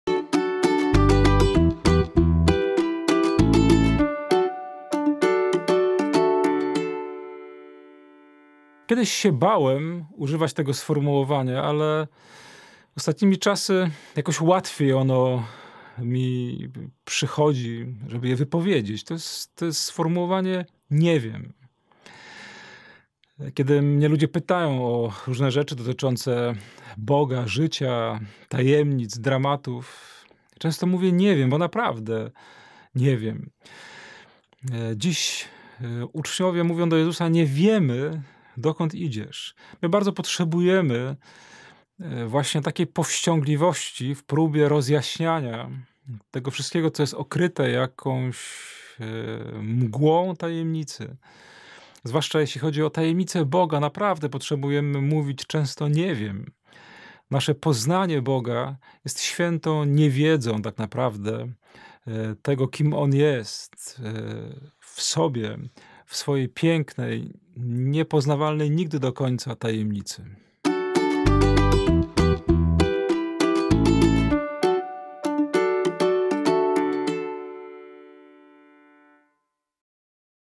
EwangeliarzOP ll 2 listopada 2019 / Msza trzecia ll J 14, 1-6
Codzienny komentarz do Ewangelii  o. Wojciecha Jędrzejewskiego OP.

Ewangelia (Łk 23, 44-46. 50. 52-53; 24, 1-6a)

Było już około godziny szóstej i mrok ogarnął całą ziemię aż do godziny dziewiątej. Słońce się zaćmiło i zasłona przybytku rozdarła się przez środek.

Wtedy Jezus zawołał donośnym głosem: «Ojcze, w Twoje ręce powierzam ducha mojego». Po tych słowach wyzionął ducha.

Był tam człowiek dobry i sprawiedliwy, imieniem Józef, członek Wysokiej Rady. On to udał się do Piłata i poprosił o ciało Jezusa. Zdjął je z krzyża, owinął w płótno i złożył w grobie, wykutym w skale, w którym nikt jeszcze nie był pochowany.

W pierwszy dzień tygodnia poszły skoro świt do grobu, niosąc przygotowane wonności. Kamień od grobu zastały odsunięty. A skoro weszły, nie znalazły ciała Pana Jezusa. Gdy wobec tego były bezradne, nagle stanęło przed nimi dwóch mężczyzn w lśniących szatach.

Przestraszone, pochyliły twarze ku ziemi, lecz tamci rzekli do nich: «Dlaczego szukacie żyjącego wśród umarłych? Nie ma Go tutaj; zmartwychwstał».